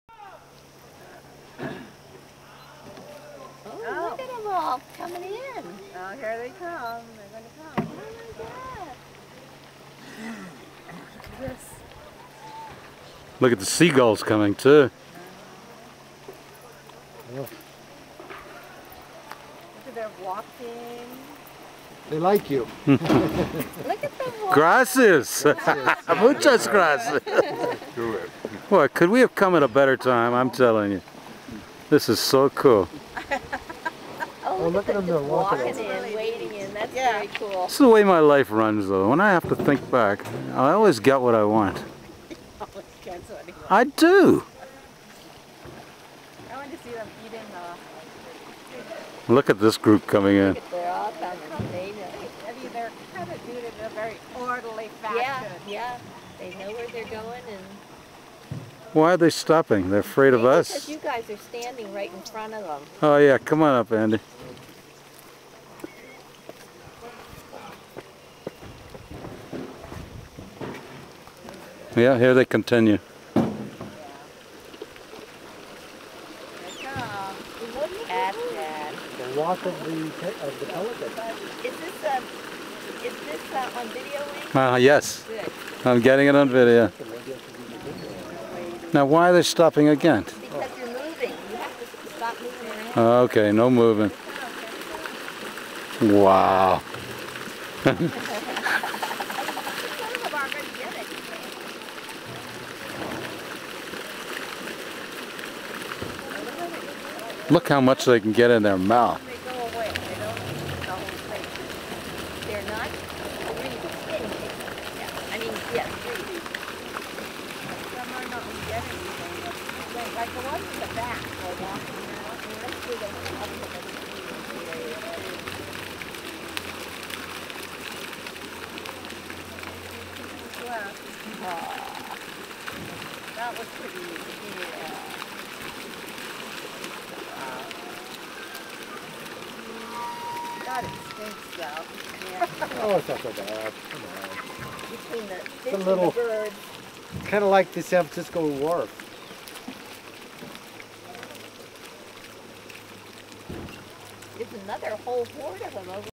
White pelicans feeding on the South shore of Lake Chapala - 1/12/2012 live
White pelicans feeding on the Souths side of Lake Chapala - 1/12/2012 live taken in Petatan Jalisco Mexico